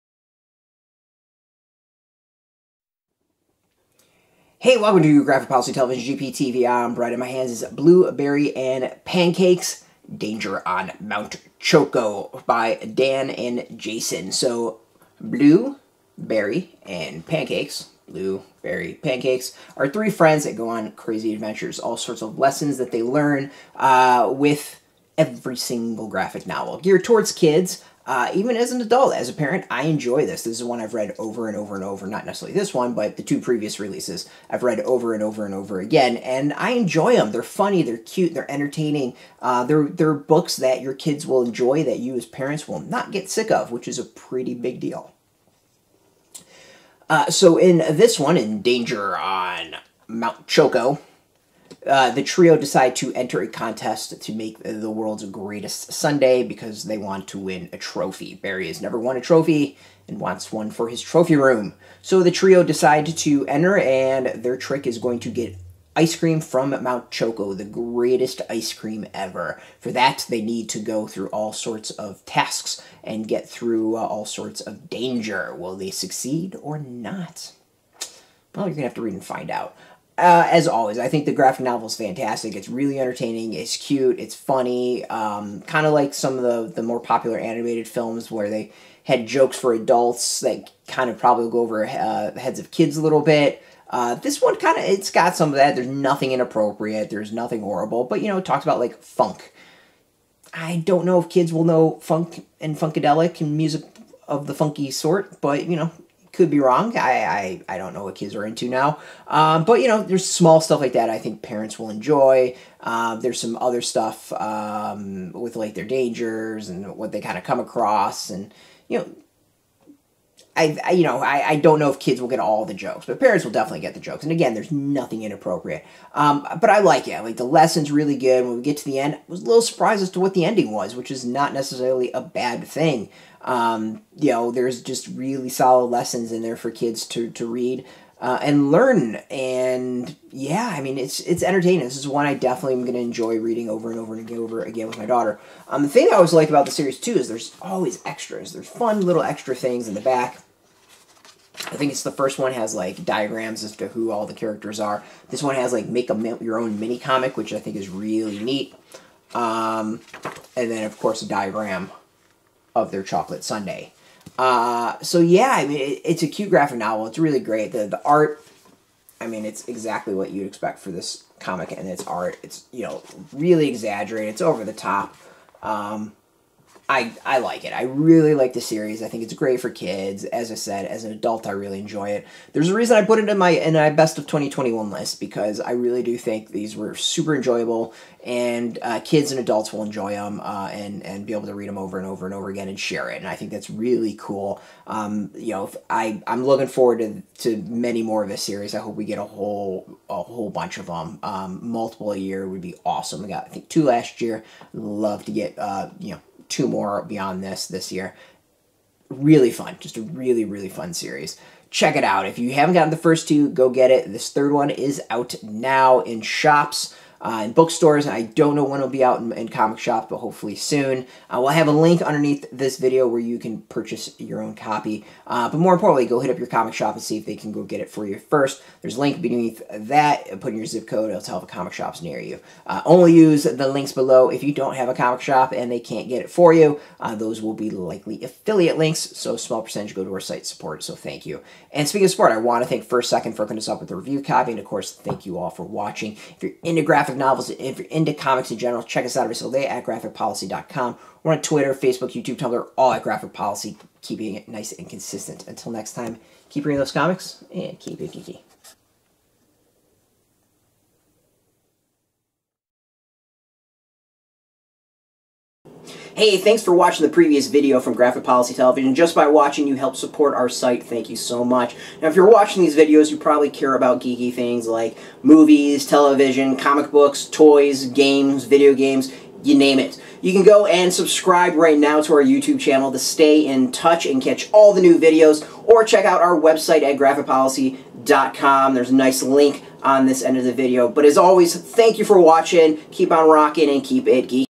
Comics Review: Blue, Barry & Pancakes: Danger on Mount Choco
Blue, Barry, and Pancakes are back for more adventure and more lessons about friendship.

Blue, Barry & Pancakes: Danger on Mount Choco continues the fantastic series full of humor, entertainment, and lessons for younger readers. This time, the trio enter a contest to make sundaes!

Story/Art: Daniel Rajai Abdo, Jason Linwood Patterson

First Second provided Graphic Policy with a FREE copy for review